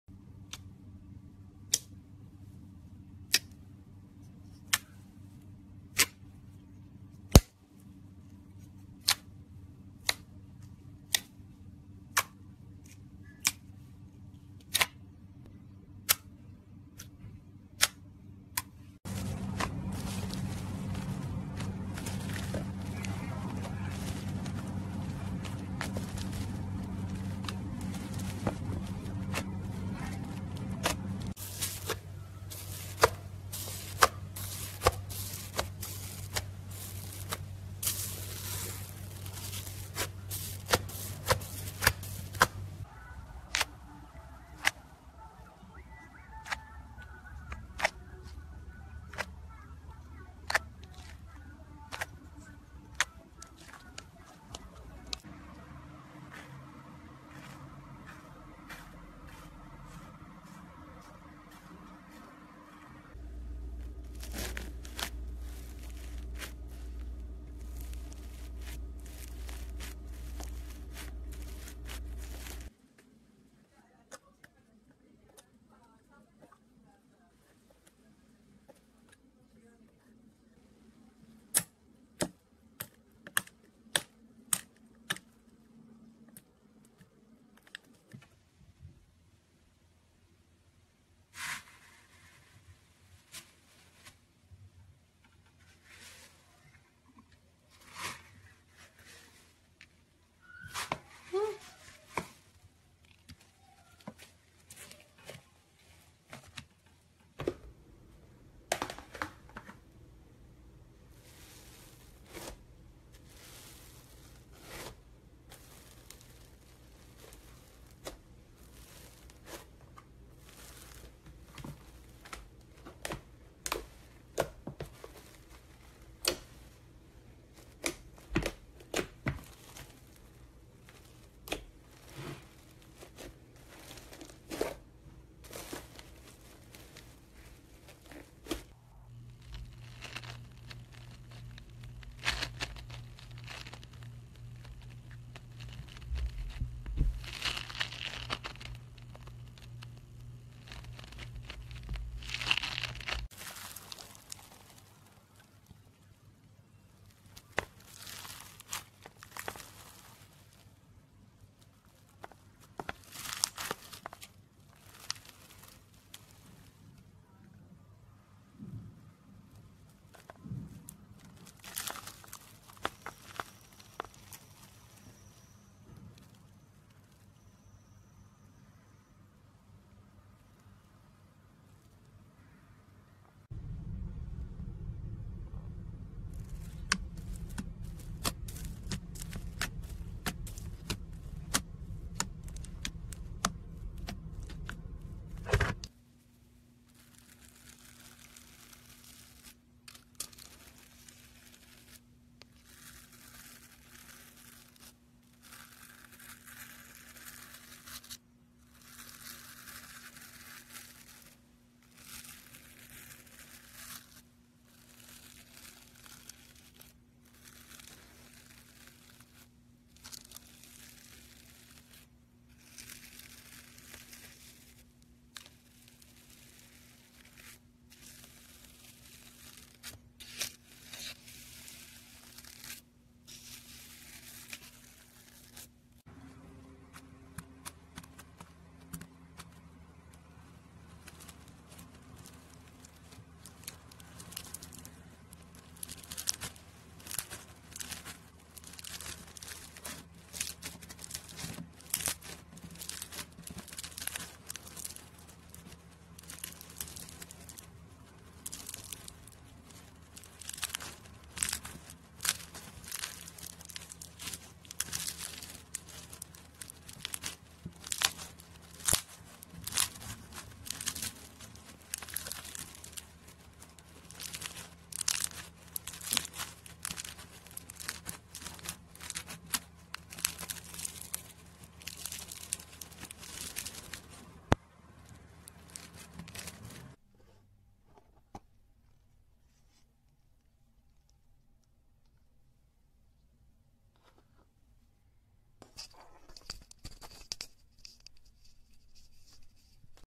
BEST Satisfying Slime ASMR Compilation Crunchy Crackle Pop Satisfy Everyone
((Turn Up The Volume For full Effect))

Satisfying Slime ASMR Video Compilation Made To Stimulate Every Single Part Of Your Body. Watch And See How These Strange Clips Keep You Staring Till The End Of The Video.